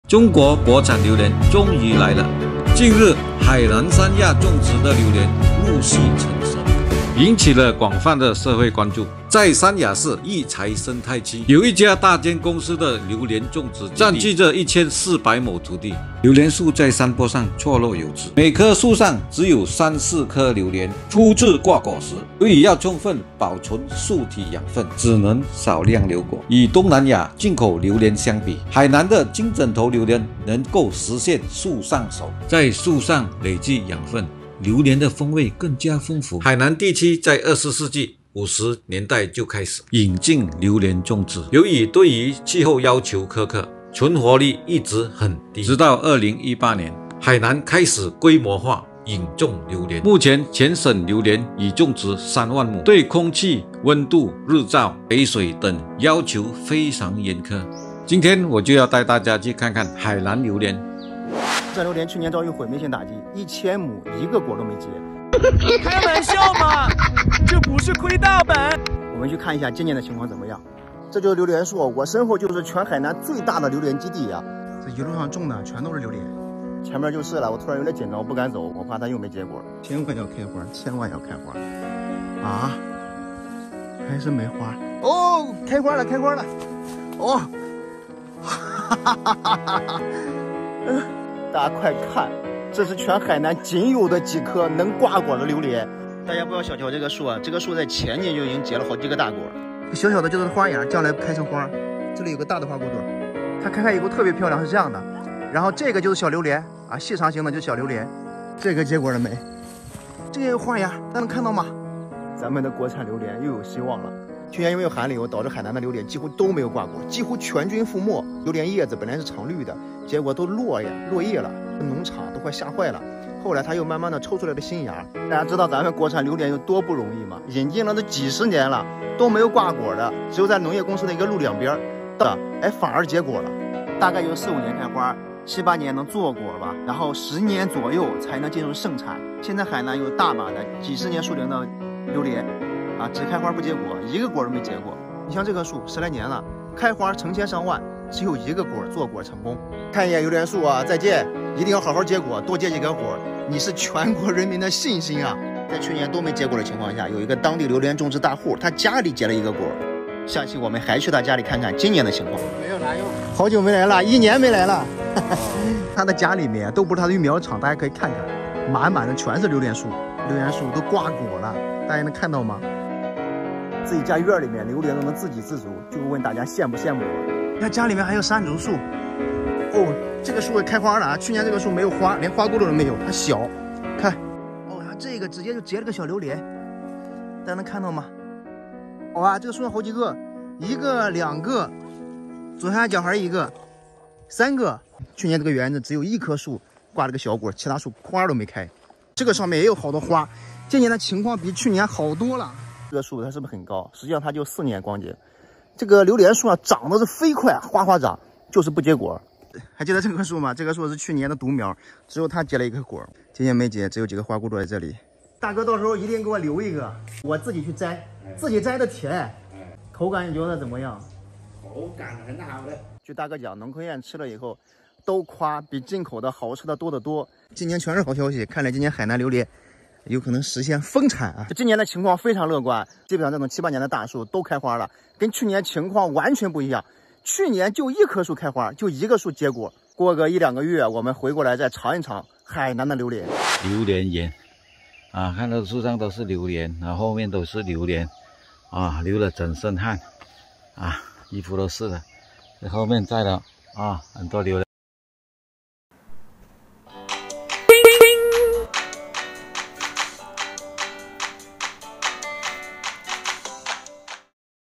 中国榴莲自由了吗？ 实地考察看一看到底真相如何？
海南榴莲1000亩榴莲树 一个果也没结？ 结果告诉我们了什么启示？

环境条件：植物的生长和果实结实通常与其所处的环境条件有关。榴莲作为热带水果，可能需要适宜的温度、湿度和土壤条件才能正常生长和结果。如果该地区的环境条件不适合榴莲生长，那么榴莲树就可能无法结出果实。

品种选择：不同品种的植物对于环境的适应性和结果能力可能不同。选择适合当地环境的品种是确保植物能够结果的重要因素。

生态平衡：植物生长需要适当的生态平衡，包括合适的传粉者（如昆虫或鸟类）来帮助传播花粉，从而促进果实的形成。如果当地生态系统遭受干扰，可能会影响榴莲树的结果。

管理和护理：榴莲树的管理和护理对于果实的形成也是至关重要的。适当的修剪、施肥、浇水和防治病虫害都可以帮助榴莲树保持健康，提高果实的产量。

气候变化：气候变化可能对植物的生长和果实结果产生影响。极端的气候事件可能导致植物受损，进而影响果实的产生。

综上所述，榴莲树1000亩没有结出果实可能是由于环境条件不适合、品种选择不当、生态平衡失衡、管理不善或气候变化等因素所致。这提醒我们在种植榴莲或其他作物时，要仔细考虑环境条件、选择适合的品种、保护生态平衡并合理管理，以提高植物的生长和结果率